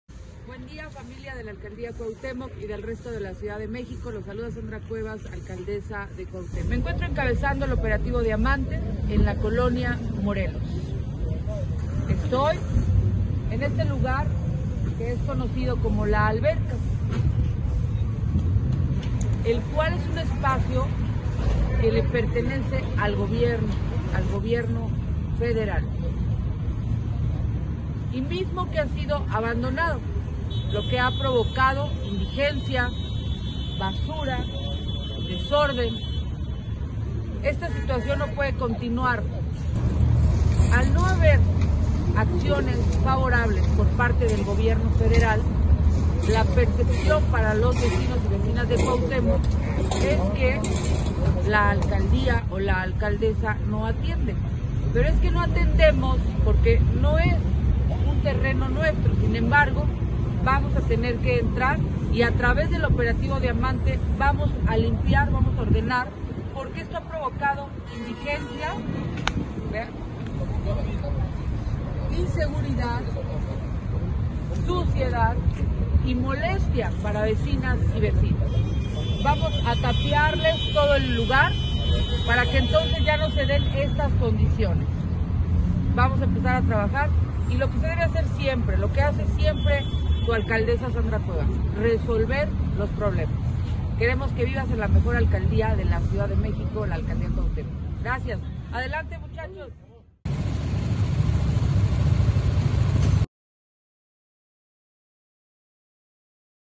En la Alcaldía Cuauhtémoc resolvemos
Aunque no nos toca ¡atendemos!🫡
💎Operativo Diamante en la 📍ColoniaMorelos.